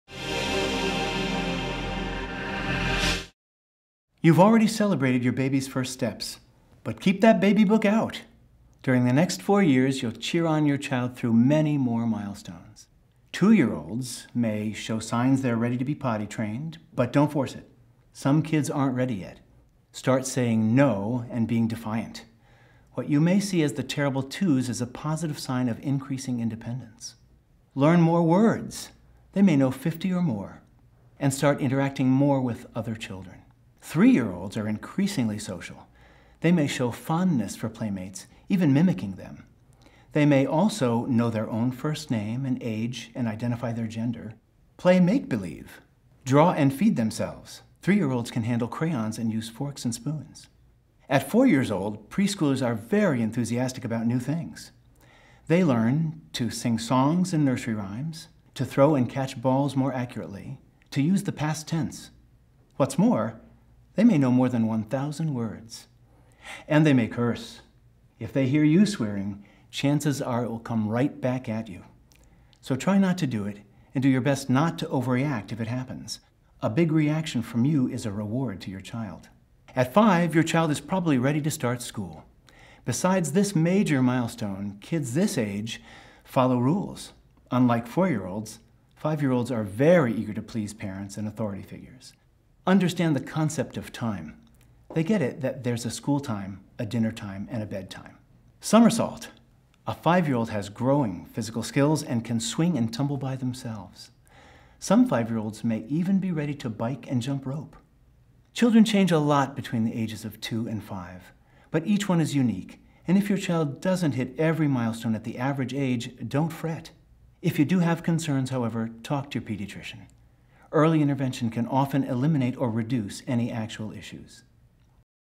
Dr. Pete discusses the Milestones of Young Children
Dr. Pete, Chief of Pediatrics at Northern Westchester Hospital in Mt. Kisco, NY, discusses the milestones of young children. There's so many milestones associated with children as they grow.  It's important to know what to expect as your baby grows up.